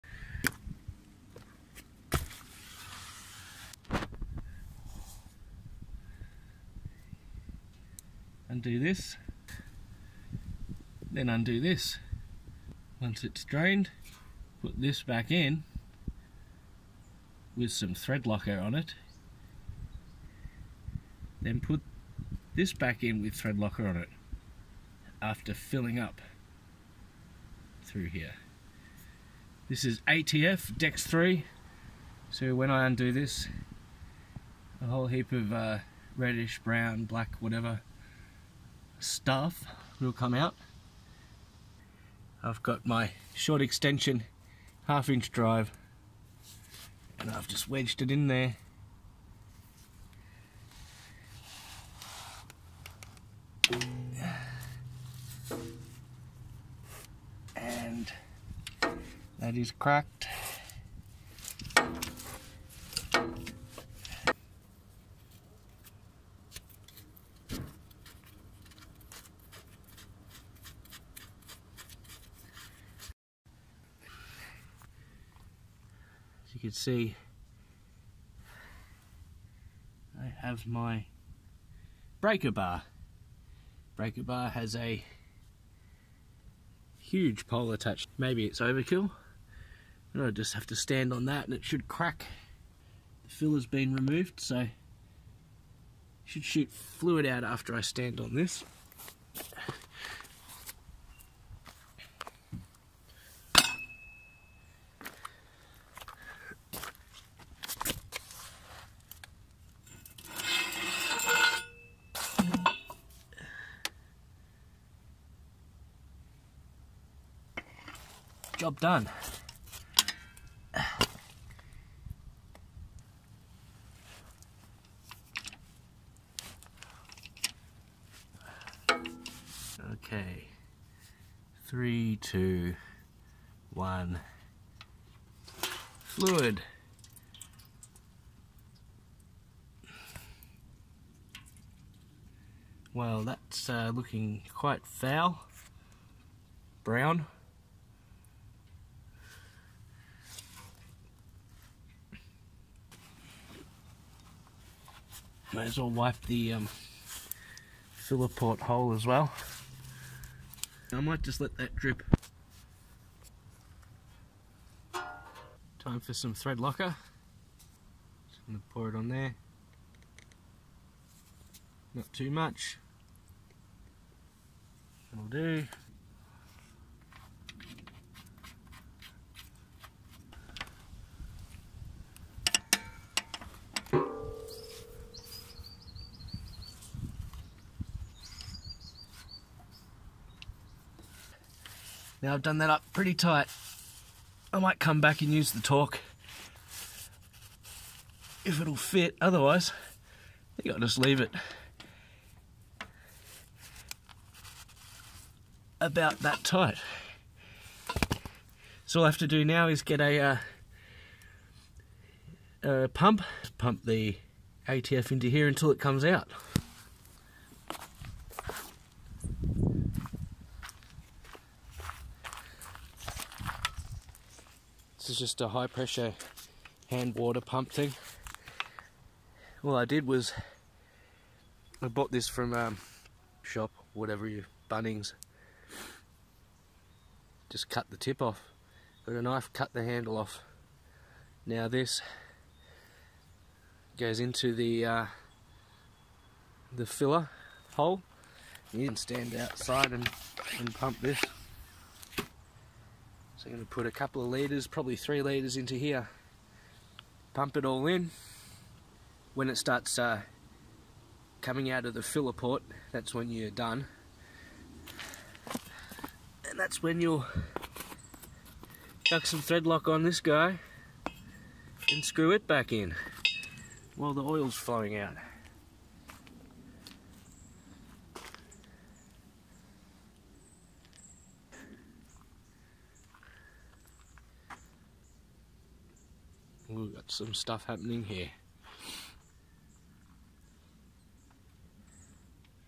P38 Range Rover 4.6 - Transfer Case Oil Change
P38 Range Rover 4.6 Thor - Transfer Case Oil Change, when car is on the ground.